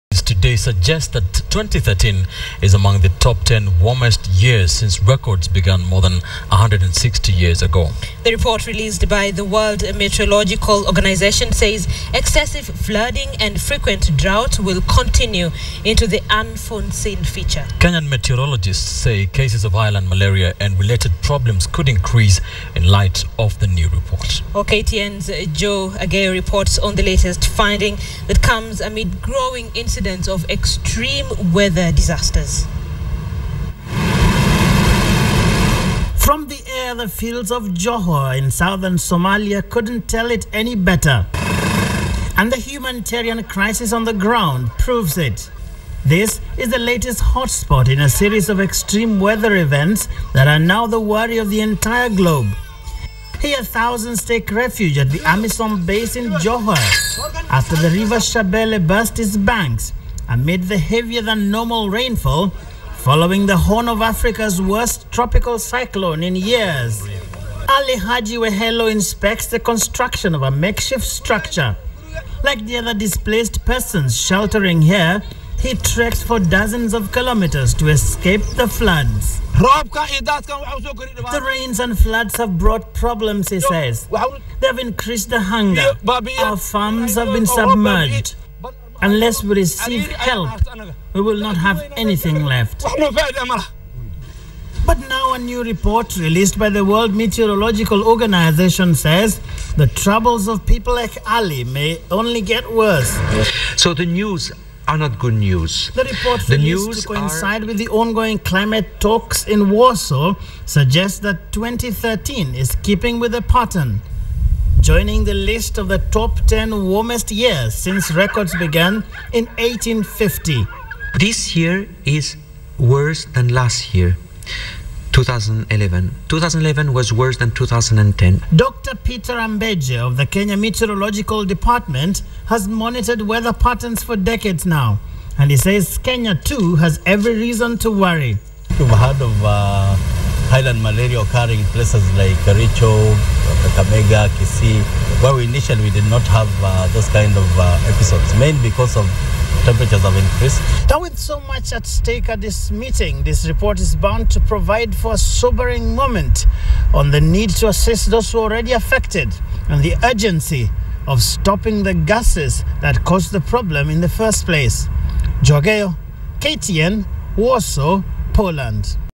Climate Change : 2013 is the top ten warmest year since 1850
A new report released today suggests that 2013 is among the top ten warmest years since records began more than 160 years ago. The report released by the world meteorological organisation says excessive flooding and frequent drought will continue into the unforeseen future. Kenya meteorologists says cases of highland malaria and related problems could increase in light of the new report.